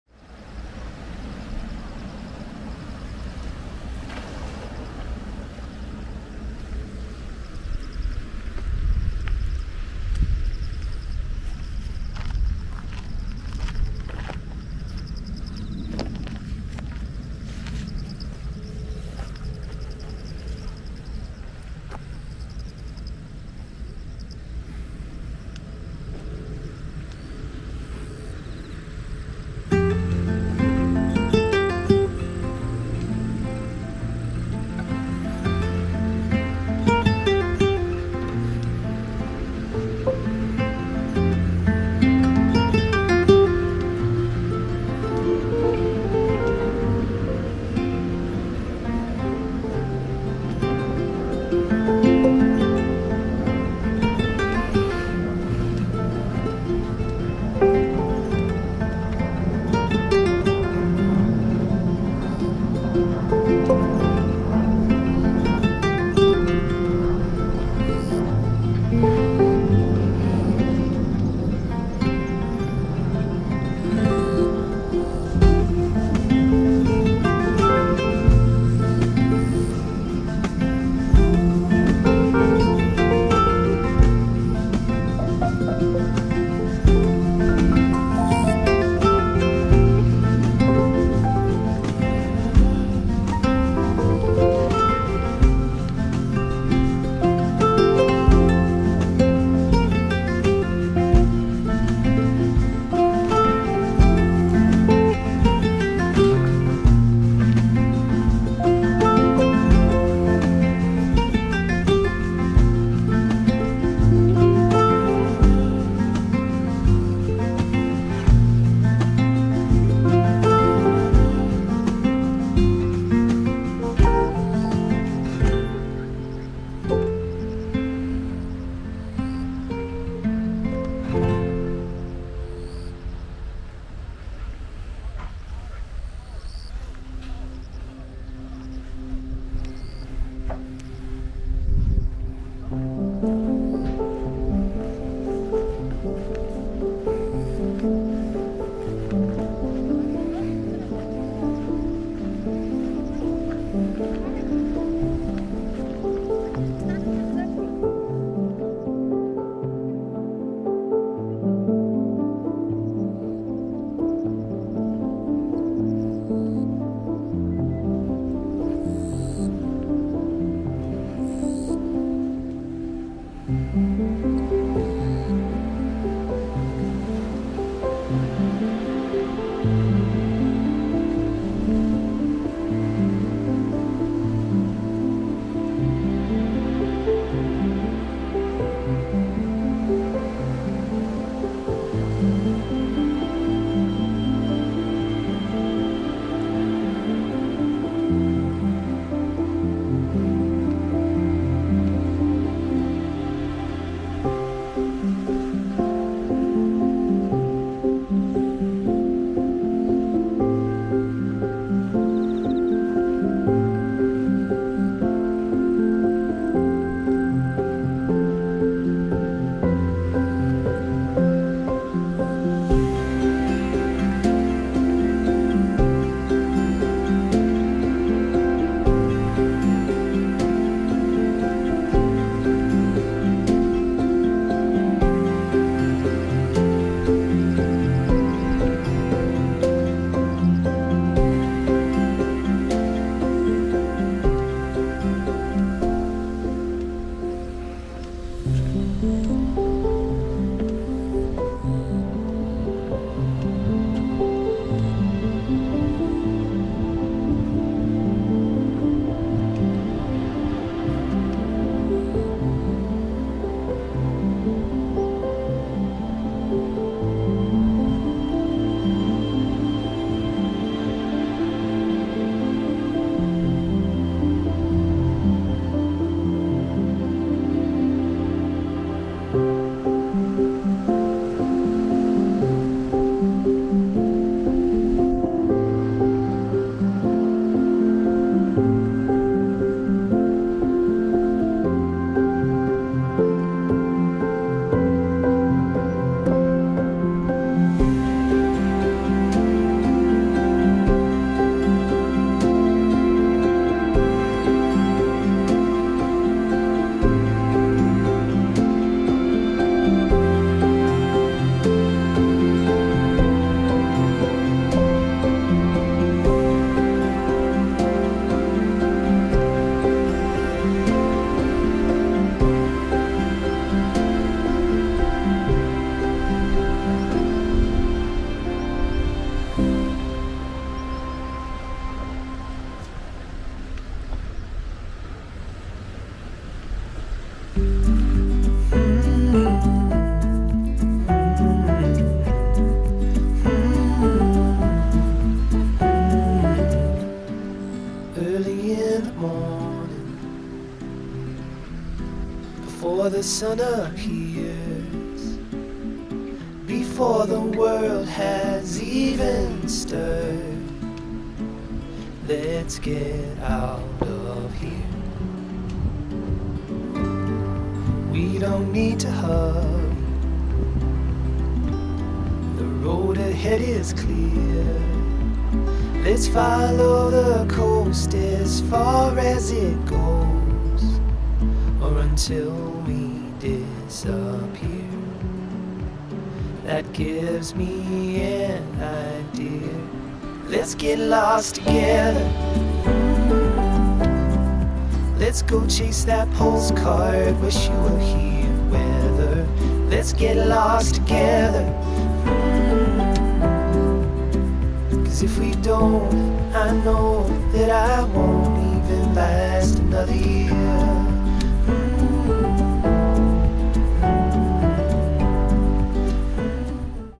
(4k & sub) Dakmeori Coastal Road on jeju in korea
Dakmeori (which translates to chicken head) Coastal Road gets its name from the fact that it looks as if a chicken is pecking the ground with its beak.

There is also a wetland called Namsang-e-mot, public bath place, and other unusual locations that make walking on Neurigil Coastal Road all the more special.

It is approximately 1.8 km long and takes 30 minutes to complete.